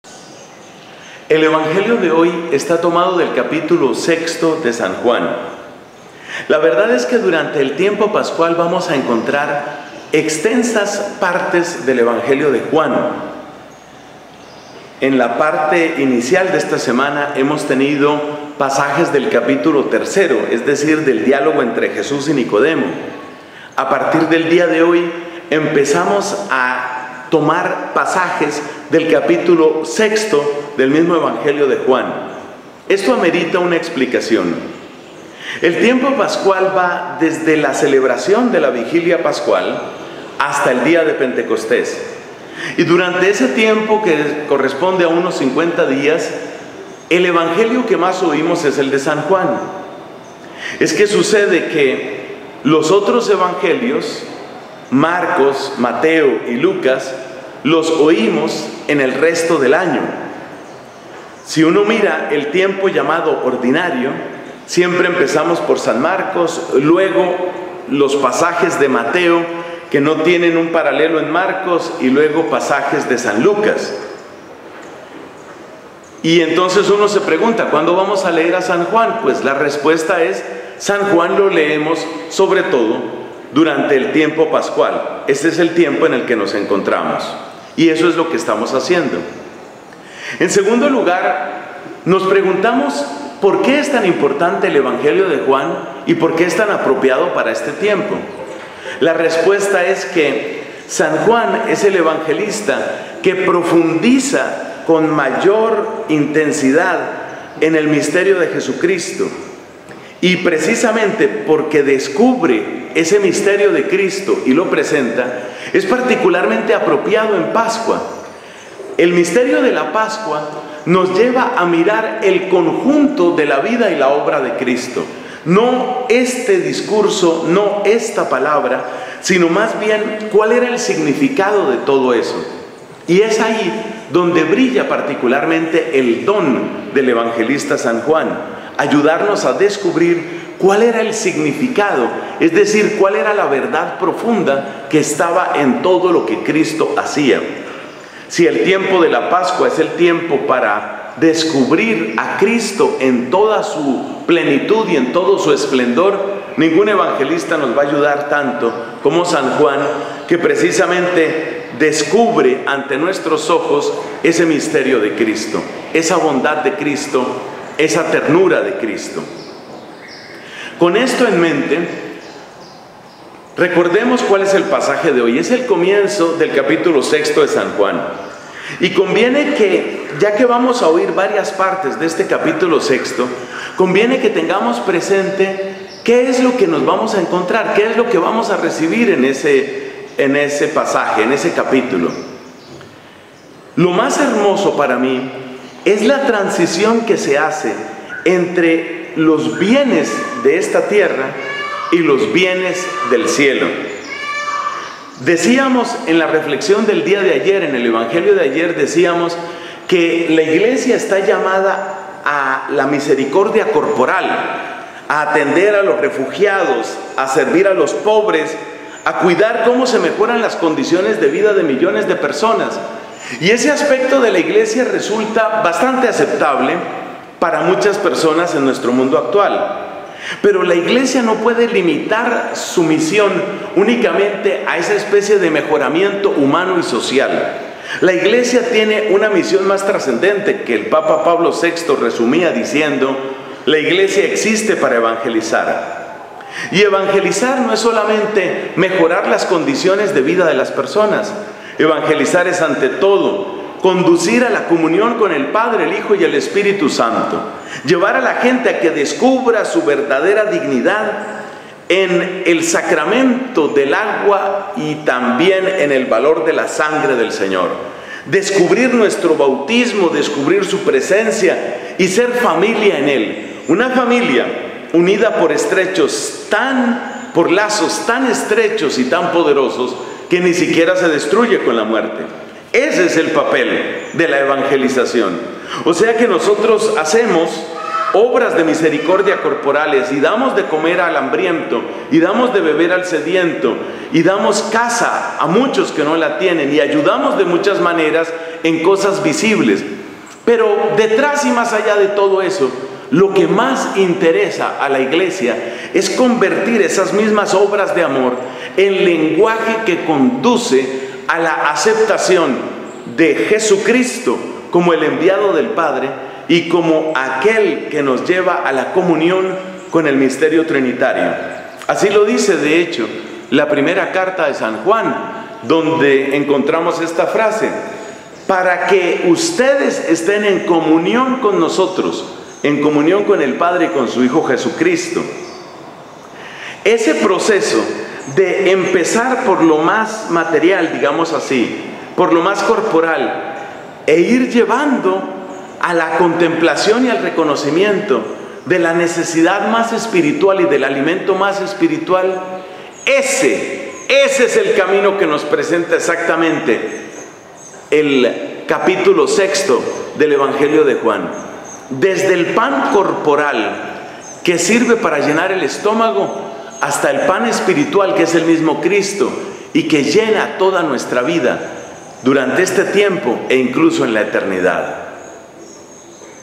LA GRACIA del Viernes 28 de Abril de 2017
Desde lo material la Iglesia nos conduce al reconocimiento de la necesidad del alimento espiritual para esta vida y la eterna, que es Cristo mismo que se nos da.